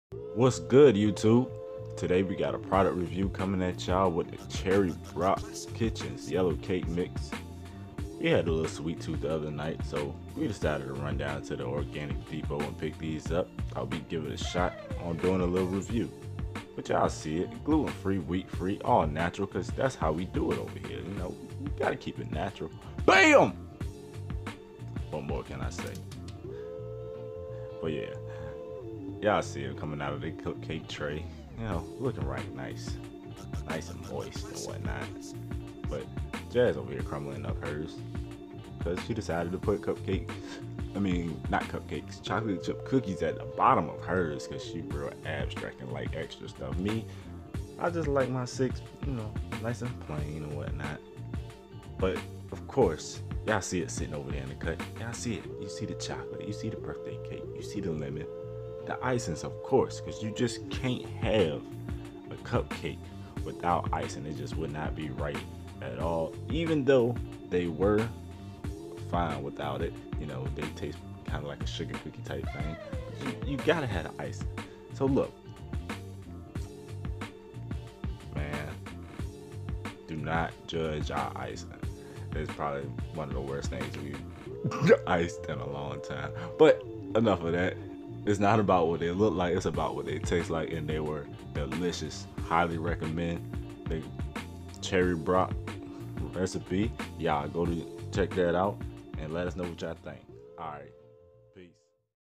VEGAN Cupcake Review: Kitchen Shenanigans!!!!!!!
While we were out, Jaz saw these and decided we would give them a shot on a review. First time for everything right? Well started off nice and ended even better. Just remember, looks can be deceiving.